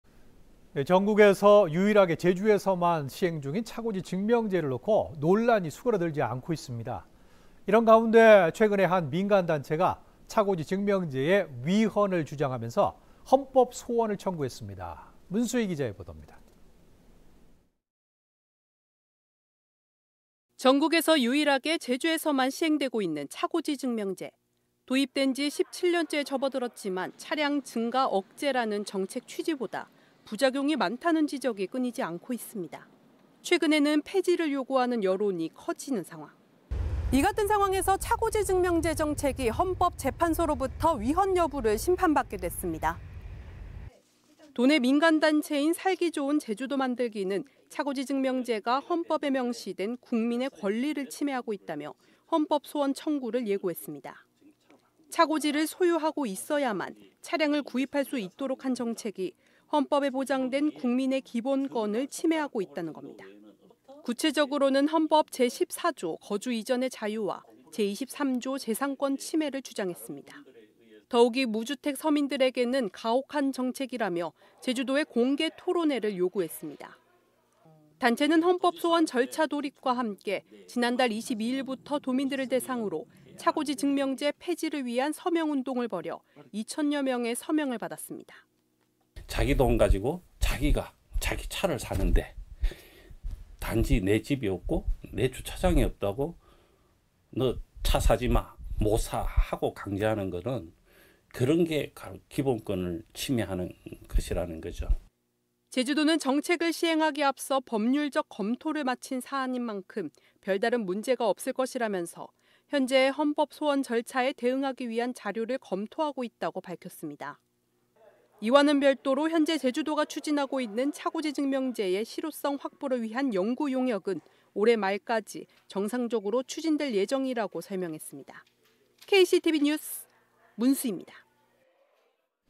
'폐지 논란' 차고지증명제 결국 헌법소원
전국에서 유일하게 제주에서만 시행중인 차고지 증명제를 놓고 논란이 수드러들지 않고 있습니다.

이런 가운데 최근 한 민간단체가 차고지증명제의 위헌을 주장하며 헌법소원 청구 절차에 돌입하기로 했습니다.

문수희 기자의 보도입니다.

전국에서 유일하게 제주에서만 시행되고 있는 차고지 증명제.

도입된 지 17년째에 접어들었지만 차량 증가 억제라는 정책 취지 보다 부작용이 많다는 지적이 끊이지 않고 있습니다.

최근에는 폐지를 요구하는 여론이 커지는 상황.

[문수희 기자]
"이런 상황에서 차고지증명제 정책이 헌법재판소로부터 위헌 여부를 심판받게 됐습니다."

도내 민간단체인 '살기좋은제주도만들기'는 차고지증명제가 헌법에 명시된 국민의 권리를 침해하고 있다며 헌법소원을 청구를 예고했습니다.

차고지를 소유하고 있어야만 차량을 구입할 수 있도록 한 정책이 헌법에 보장된 국민의 기본권을 침해하고 있다는 겁니다.

구체적으로는 헌법 제14조 거주이전의 자유와 제23조 재산권 침해를 주장했습니다.

더욱이 무주택 서민들에게는 가혹한 정책이라며 제주도에 공개 토론회를 요구했습니다.

단체는 헌법소원 절차 돌입과 함께 지난달 22일부터 도민들을 대상으로 차고지증명제 폐지를 위한 서명 운동을 벌여 2천여 명의 서명을 받았습니다.

[허민호 / 살기좋은제주도만들기 회장]
"자기 돈을 가지고 자기 차를 사는데 단지 내 집이 없고 내 주차장이 없다고 "너 차 사지마, 못 사"하고 강제하는 것은 기본권을 침해하는 것 입니다."

제주도는 정책을 시행하기 앞서 법률적 검토를 마친 사안인 만큼 별다른 문제가 없을 것이라면서 현재 헌법소원 절차에 대응하기 위한 자료를 검토하고 있다고 밝혔습니다.

이와는 별도로 현재 제주도가 제주도가 추진중인 차고지증명제의 실효성 확보를 위한 연구 용역은 올해 말까지 정상적으로 추진될 예정이라고 설명했습니다.

(영상취재 : 박병준 , 그래픽 : 유재광)